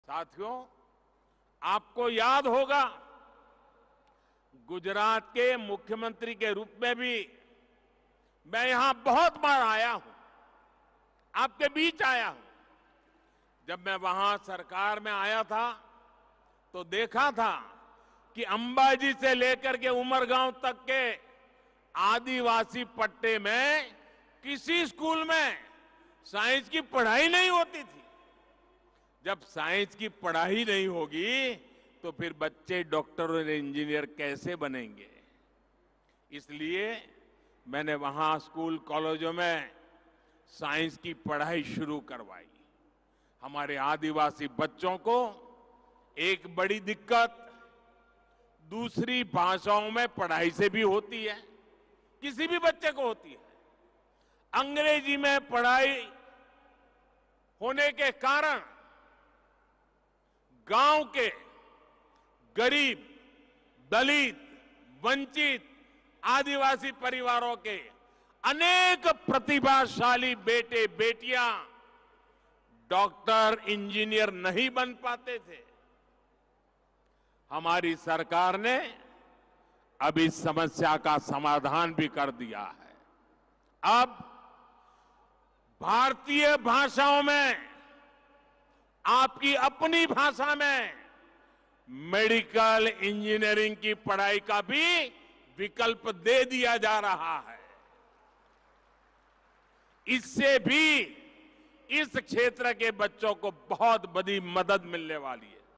Gujarat के मुख्यमंत्री रहते Modi Ji ने ऐसे की थी दमन, दीव, दादरा और नगर हवेली की मदद!
PM Modi laid the foundation stone and dedicated to the nation various development projects worth more than Rs 4850 crores at Silvassa, Dadra, and Nagar Haveli today. The projects included the dedication of NAMO Medical Education & Research Institute in Silvassa, and the laying of the foundation stone of 96 projects